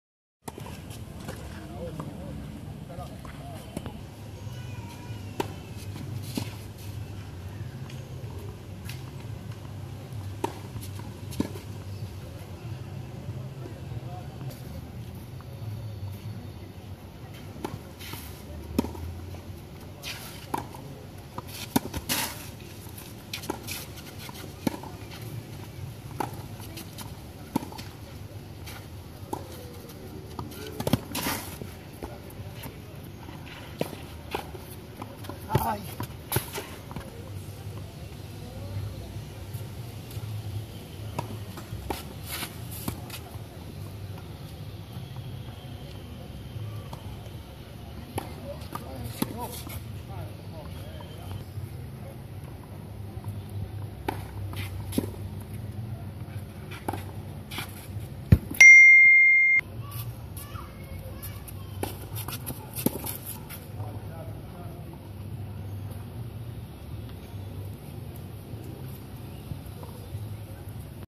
CARLOS PIEDRAS - COPA ANDY 2016 - TENIS BERAZATEGUI - CATEGORIA SUPER AVANZADA
sede : ex club ducilo